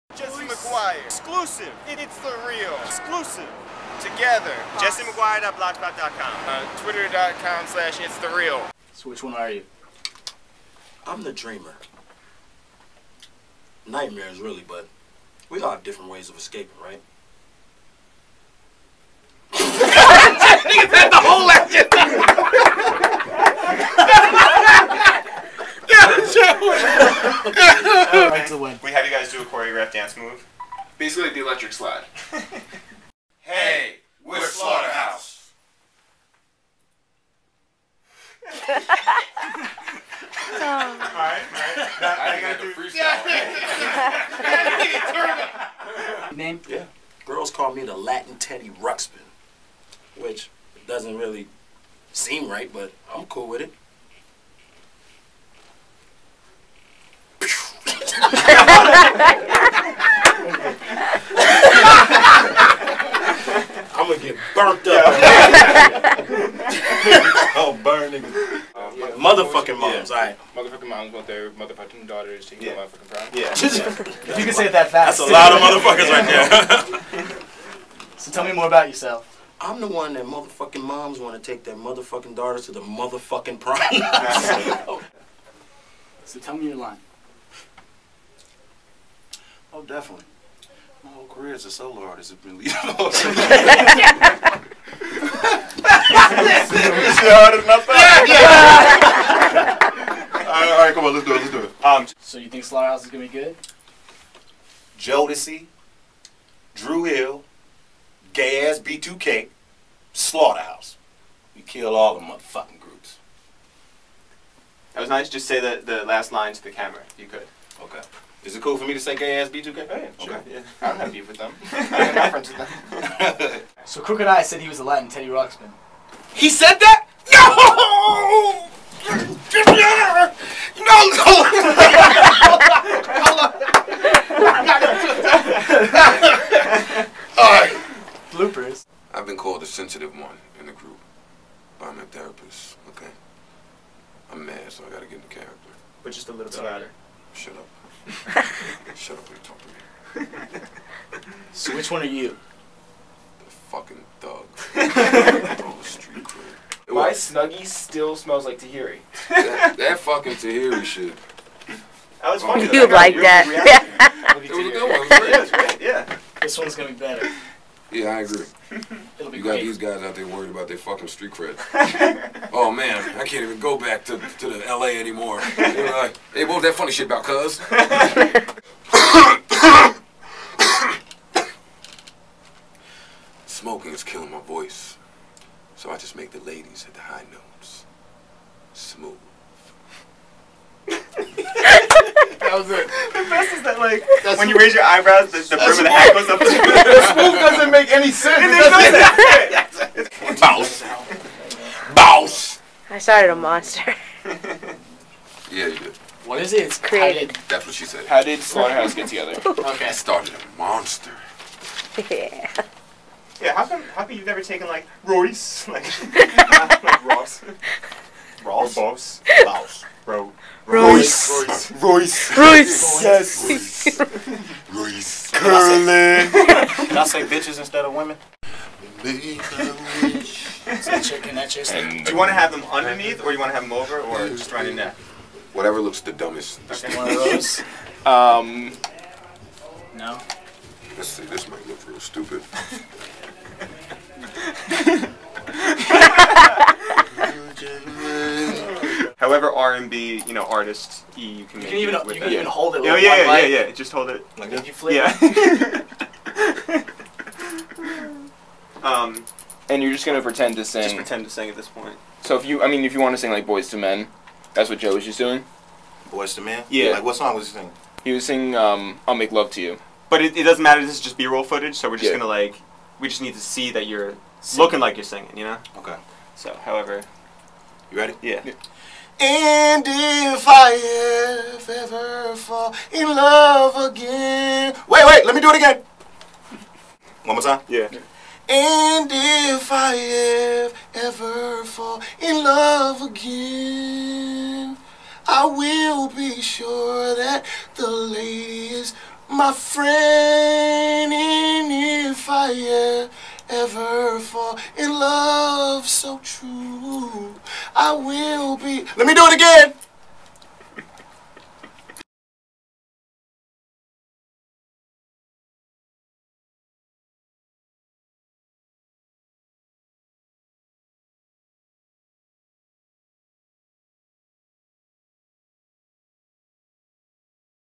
Jessie Maguire Presents: Behind the scenes with slaughterhouse and itsthereal part 1
behind the scenes of itsthreal.com 's new video: Slaughterhouse rules